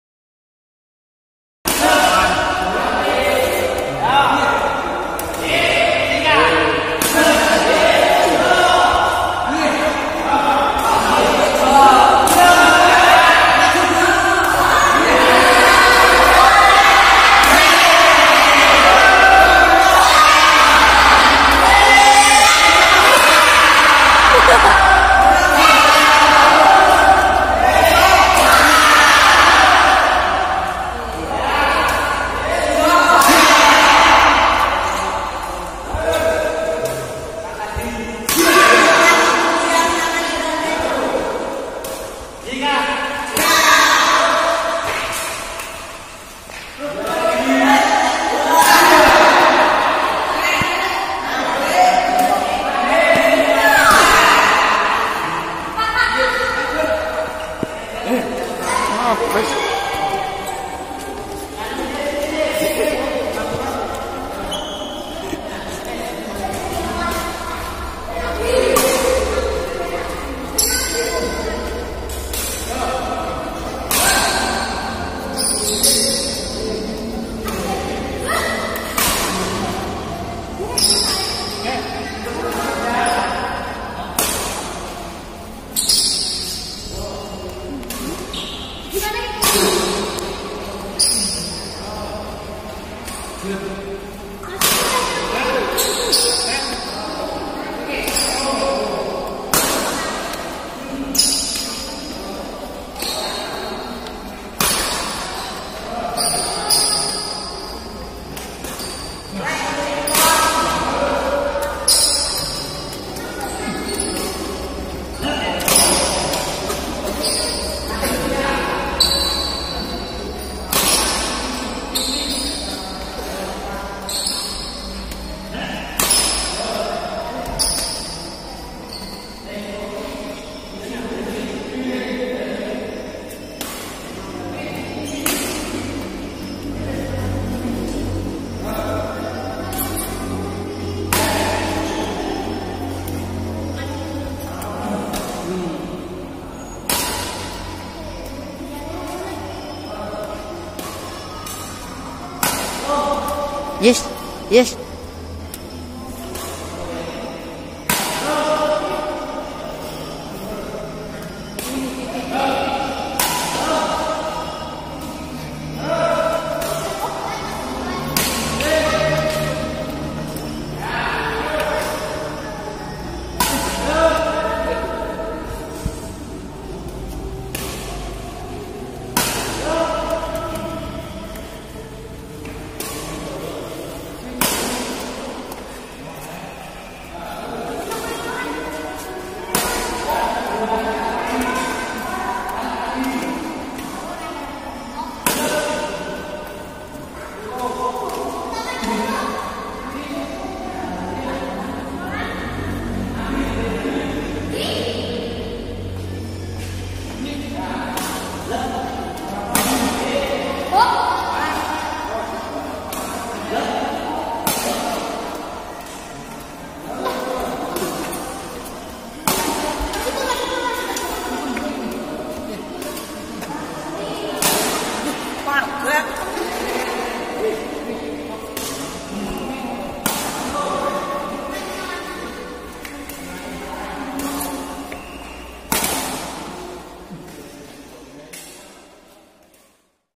Pelatihan Pembinaan bimbingan bulutangkis PB GARUDA tingkat Pelajar
Pelatian setruk fisik power pukulan & langkah PB GARUDA Lubang Sampang unt kekuatan Bulutangkis .